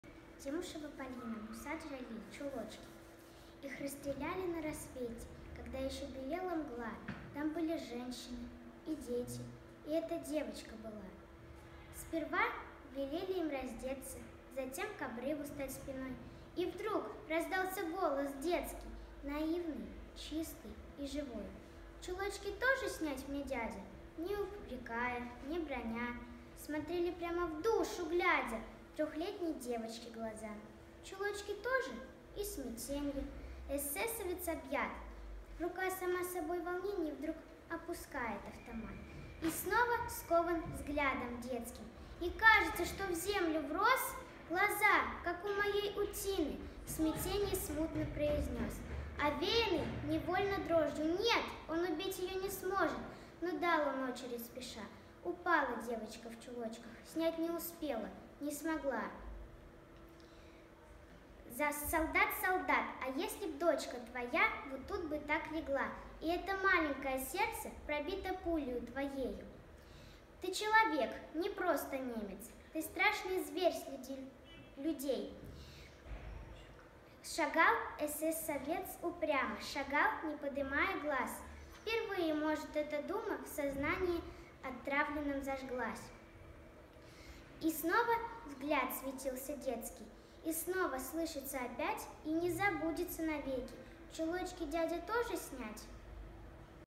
30 Демушева Полина, школа 23, класс 3 Б М Джалиль Чулочки
Городская литературно—художественная онлайн акция
«За строкой летит строка»